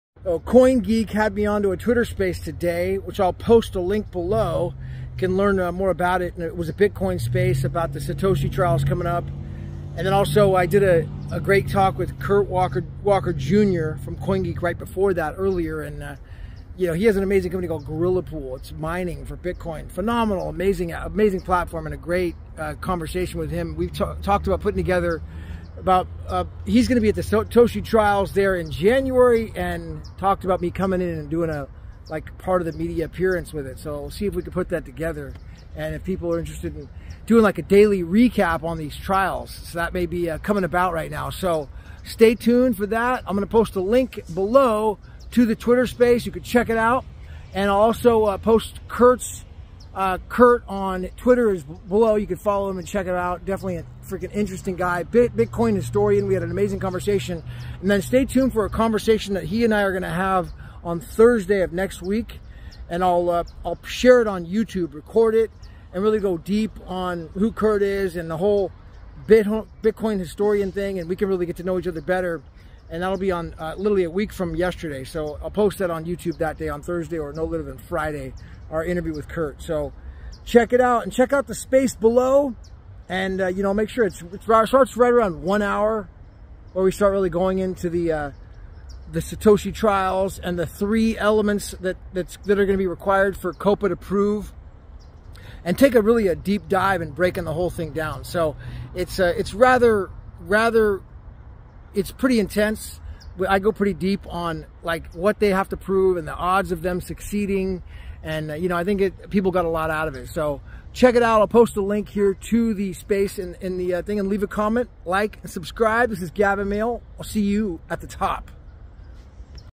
Coingeek X Space: The Bitcoin Satoshi Trials - A Meeting With The Man Who Knows Everything
Coingeek Twitter Space Starts at about 1:05